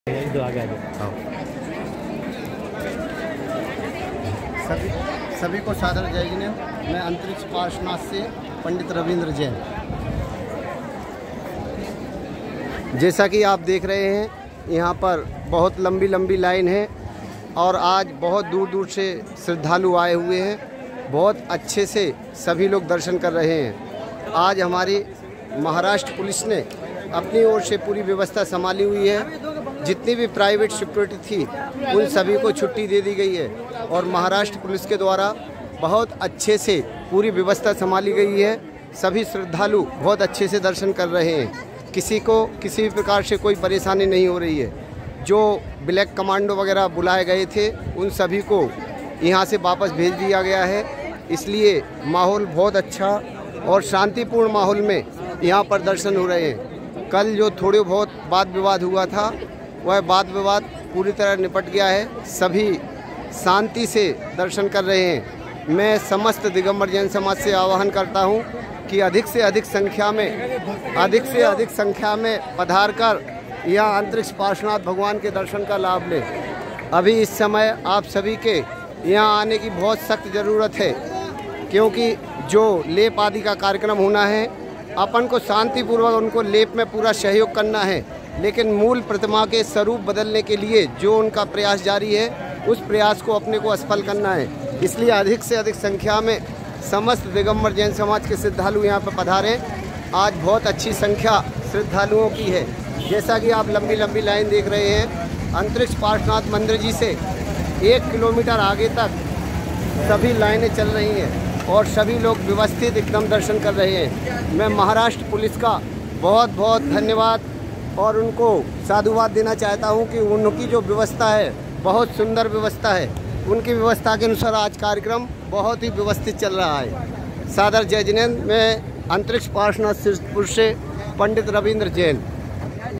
अंतरीक्ष पार्श्वनाथ शांतीपूर्ण वातावरणात दर्शन सुरू पं.रवींद्र जी जैन
*🛕श्री अंतरिक्ष पार्श्वनाथ जी मंदिर, शिरपुर जैन में आज १९/०३/२०२३, रविवार को हजारों की संख्या में श्रावक शांति से कतारबद्ध लगभग एक किलोमीटर लाइन लगाकर श्री अंतरिक्ष पार्श्वनाथ भगवान का दर्शन कर रहे हैं* 🙏🙂

*➡️दर्शनार्थियों और क्षेत्र की सुरक्षा व्यवस्था अब केवल  महाराष्ट्र पुलिस द्वारा संभाली जा रही है एवं सभी प्राइवेट सिक्युरिटी (बाउंसर) को क्षेत्र से हटा दिया गया है*

*📹पंडित रविन्द्र जी जैन द्वारा रिकॉर्ड किए गए आज १९/०३/२०२३ रविवार दोपहर को क्षेत्र पर दर्शनार्थियों की लंबी कतारों के सलग्न २ वीडियो अवश्य देखें*

*🛕श्री अंतरिक्ष पार्श्वनाथ मंदिर, शिरपुर जैन, जिला वाशिम, महाराष्ट्र मंदिर जी की गूगल मैप लिंक*
Shri 1008 Antriksh Parshwanath Digambar Jain Atishay Kshetra , Shirpur Jain

 *⛳शिरपुर जैन पहुँचने के लिए महाराष्ट्र के वाशिम जिले में मालेगांव जहांगीर निकटतम गांव है जहां से शिरपुर मात्र 11 km है*

*⛳वाशिम से शिरपुर 32 km है*

*🪷जिनशासन जयवंत हो🪷*

    *🛕जिनशासन संघ🛕*